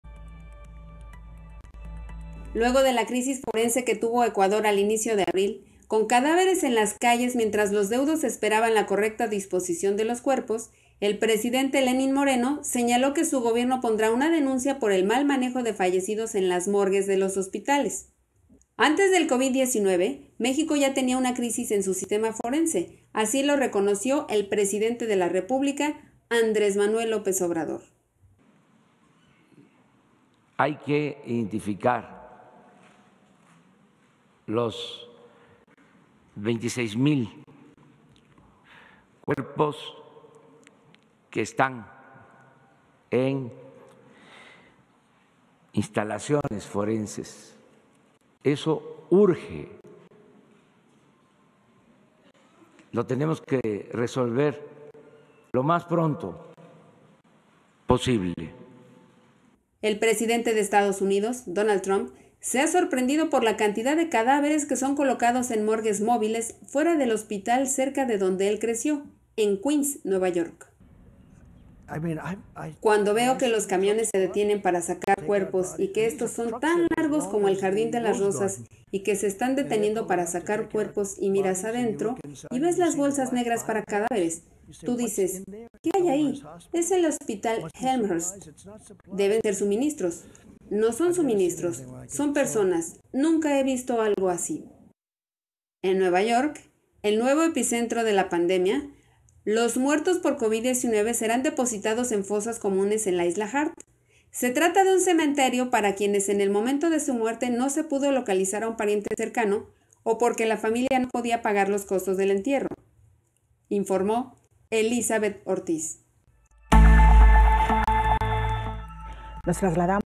Los gobiernos definen protocolos para la disposición de los cuerpos de los fallecidos por
Ecuador, México, Estados Unidos, reconocen una crisis en el manejo de los cuerpos de fallecidos por VOCI19 que permanecían en instalaciones forenses.

Un reporte de Elizabeth Ortiz